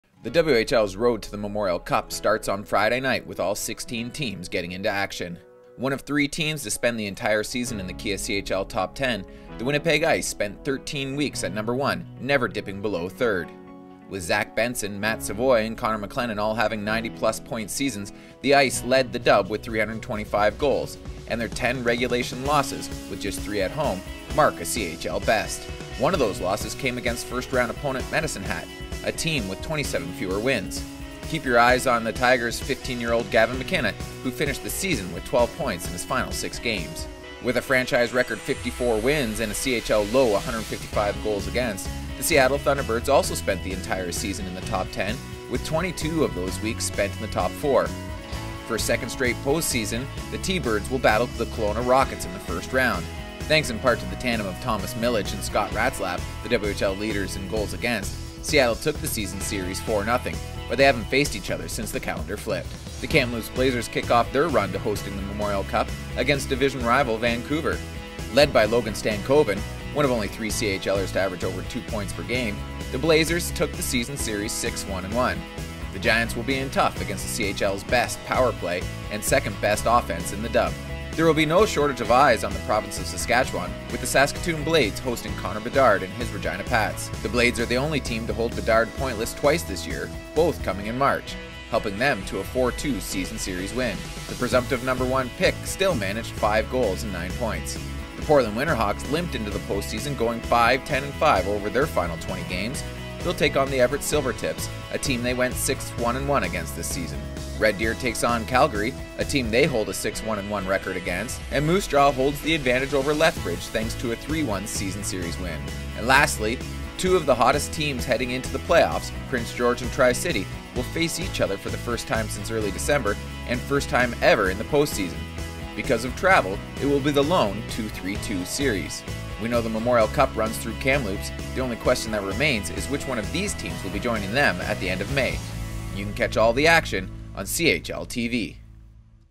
The WHL's Road to the Memorial Cup is ready to start!
We know the Memorial Cup runs through Kamloops the only question that remains is which one of these teams will be joining them at the end of May.